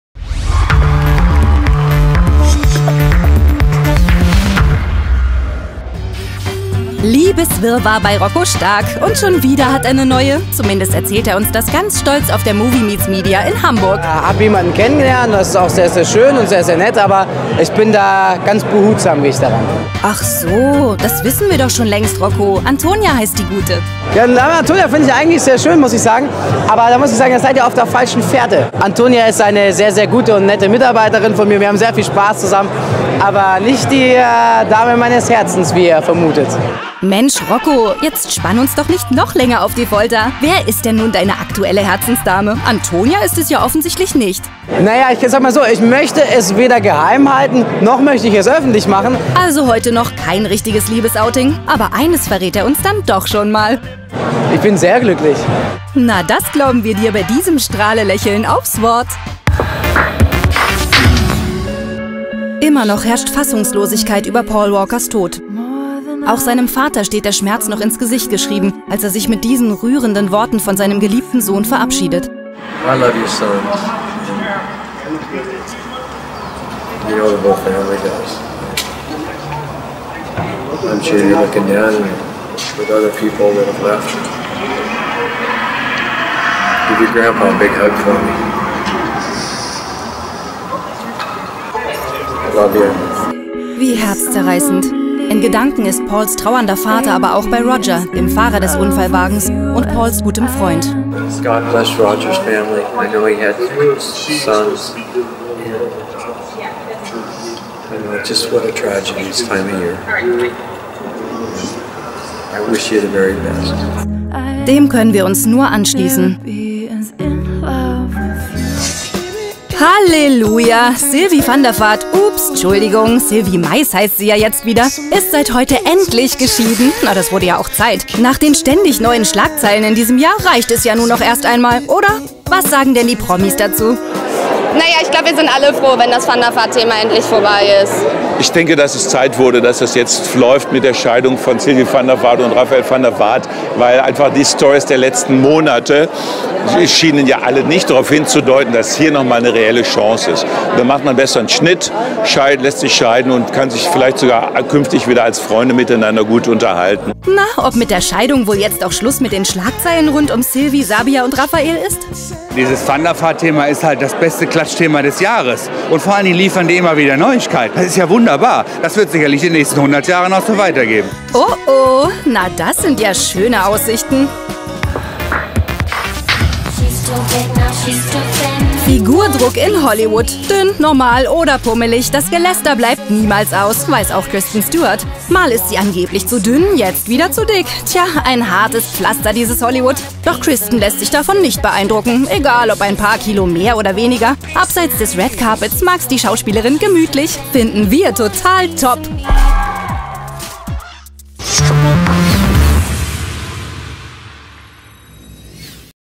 Doch nicht Antonia! Rocco Stark schwärmt vom neuen Liebes-Glück
Rocco Stark spricht über seine neue Liebe, Paul Walkers Vater nimmt Abschied von seinem Sohn, Sylvie van der Vaart ist nun geschieden, Kristen Stewart im Kilo-Check